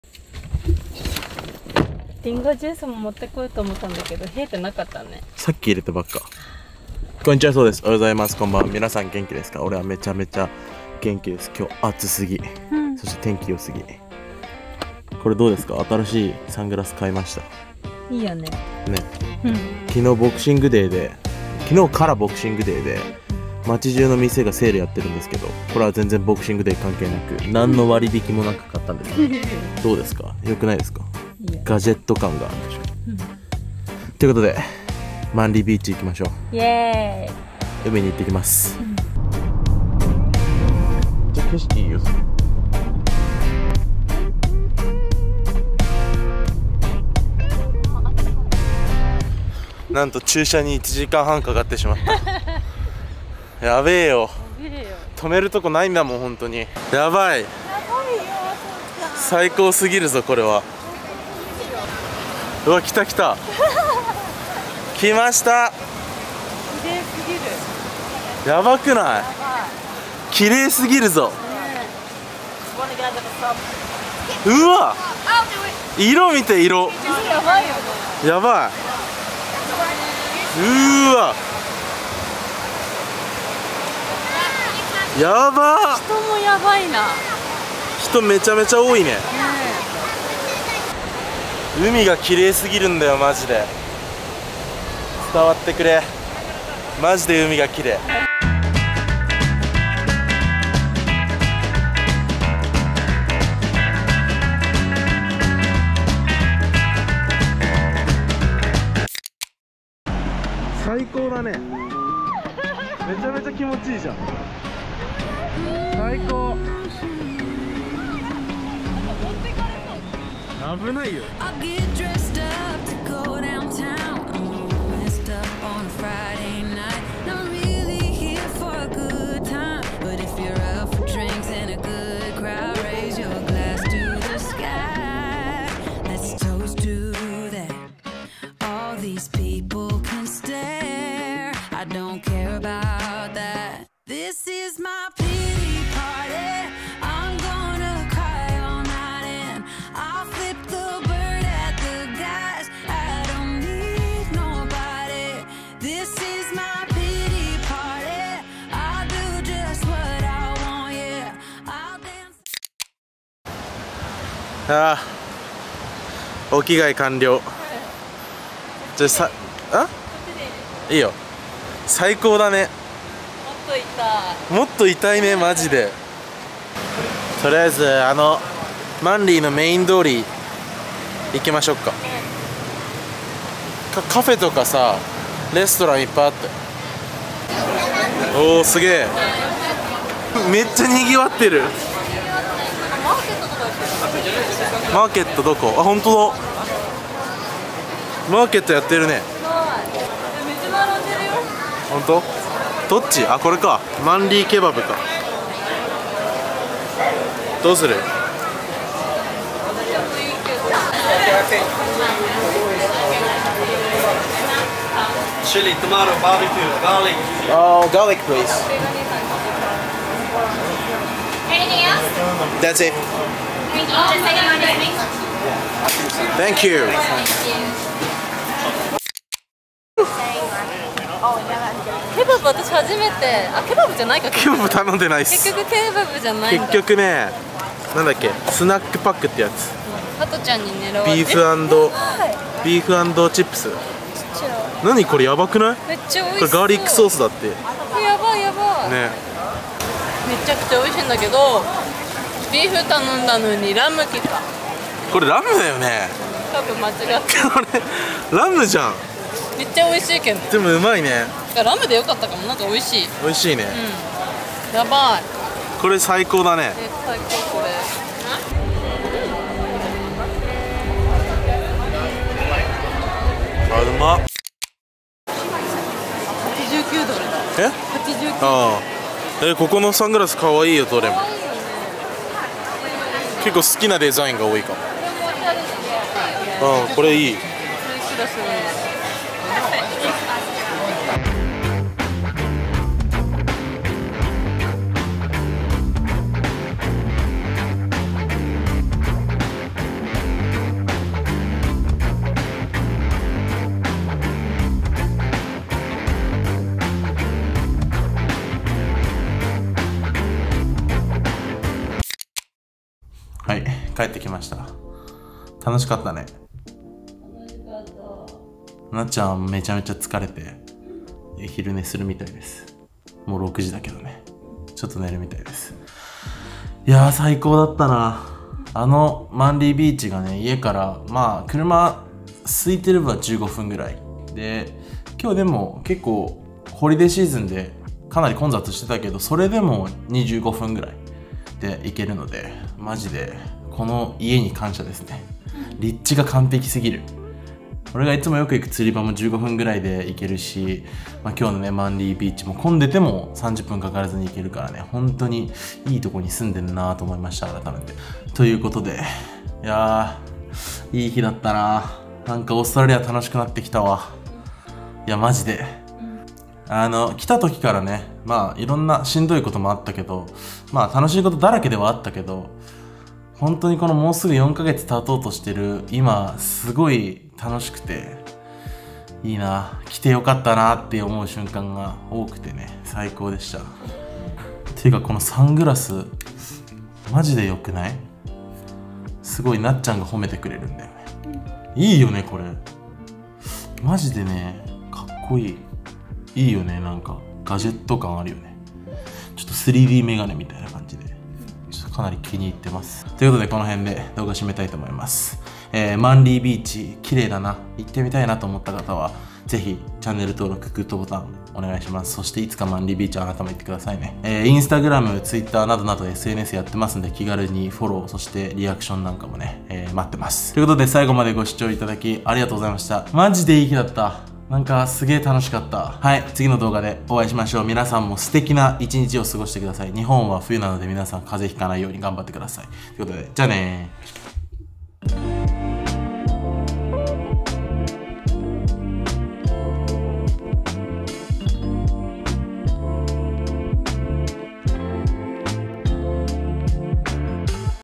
【EP.45】シドニーの大人気ビーチが家から15分で行けちゃいます inマンリービーチ | オーストラリアワーホリVLOG
▶︎今日の日記
よく行く釣具屋は家から5分。
通ってる釣り場は家から15分。
ショッピングモールは家から10分。
ホームセンター家から3分。
ガソスタは家から2分。
そしてマンリービーチは家から15分。
恵まれすぎ。